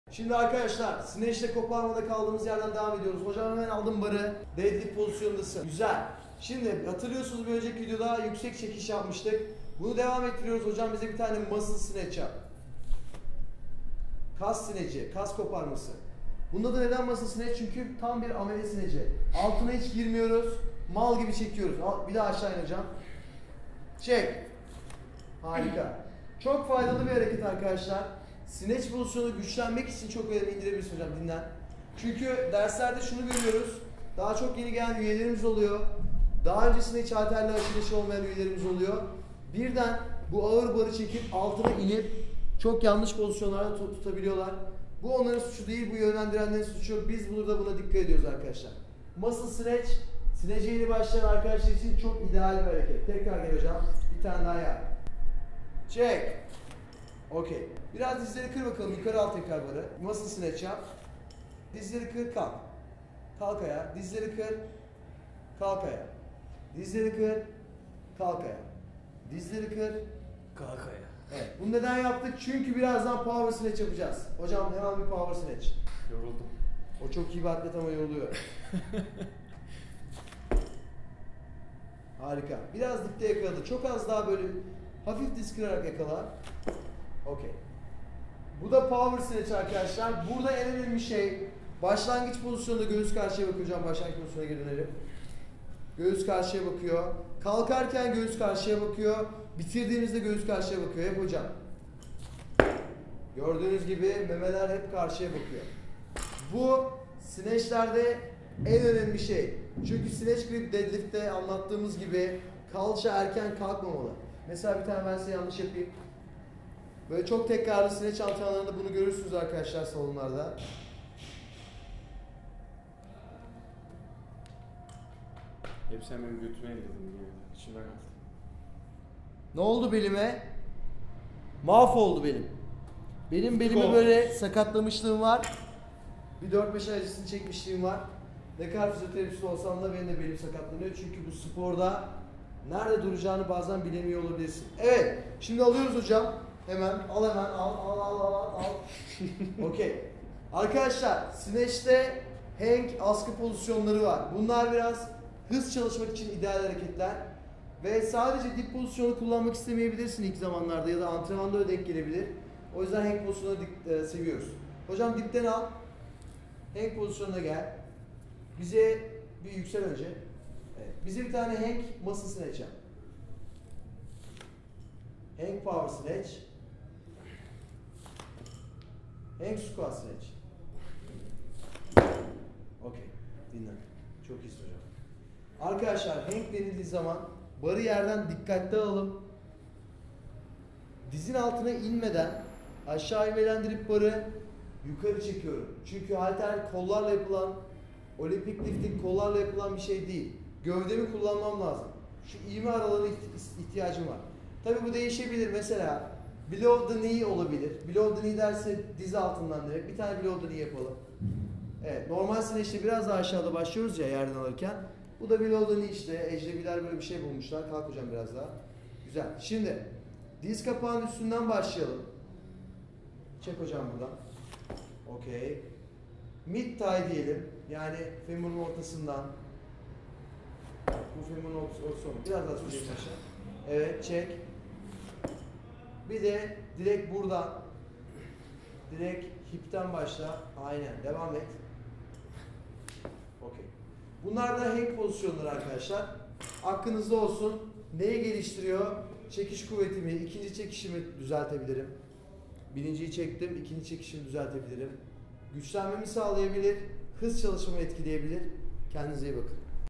FitKAFA Hareket Kütüphanesi - Snatch 2 (Koparma 2)
Muscle Snatch (Kas Koparması), Power Snatch(Güç Koparması), Hang Pozisyonları(Askı Pozisyonları)